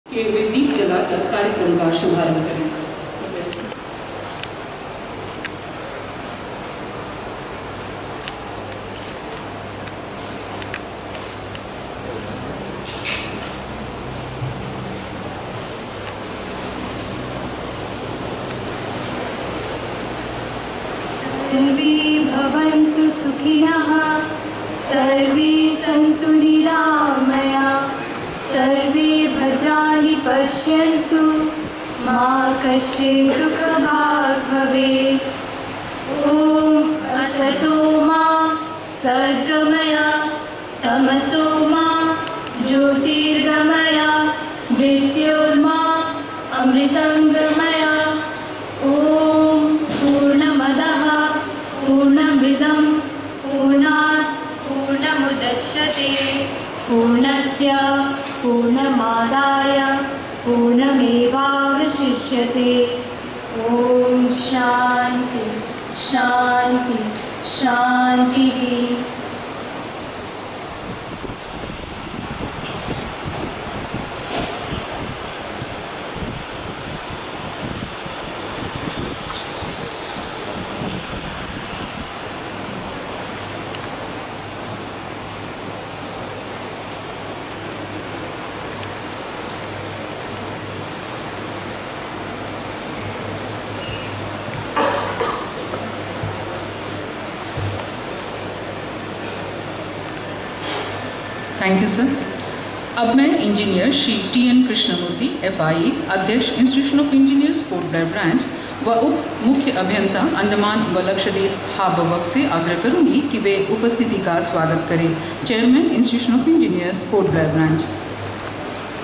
Shri ArunBarokaIAS ChiefGuest IEIA&NSC celebrates 48thErs’DayEdweepNews3
Shri.Arun –Baroka-IAS-Principal Secretary (PWD) A&N Admn-Chief-Guest-Institue of Engineers (India)Andaman & Nicobar State Centre -celebrates-48thEngineers’Day – conferred “Best Engineer 2015 Award ‘ -T he Project Director ASTRA, DRDO, Er. Sankaran Venugopal, graced Guest of Honour, Chairman Er.T.N.Krishnamoorthy  Dy.CE welcomed the gathering and Er.Usha Honorary Sec. reported brief about the annual activities of IE.(india) A&N Statae Centre.(Please visit more information to-morrow AM Publicataions)